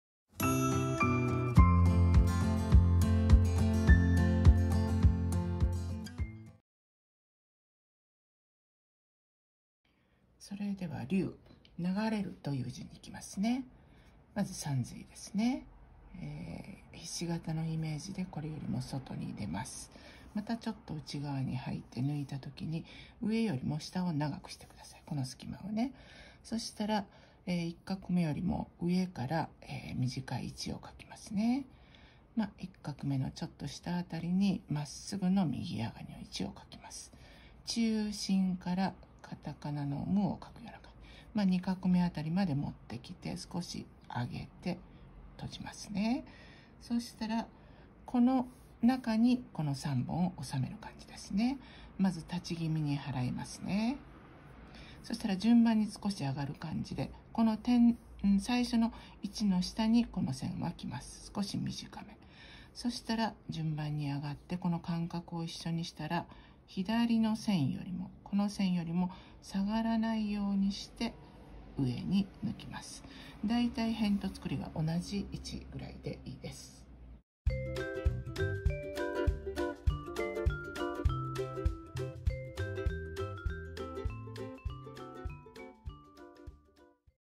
流の書き方
意識するところ　コツを知るだけで文字は変わっていきます
一緒にレッスンして美文字を目指していきましょう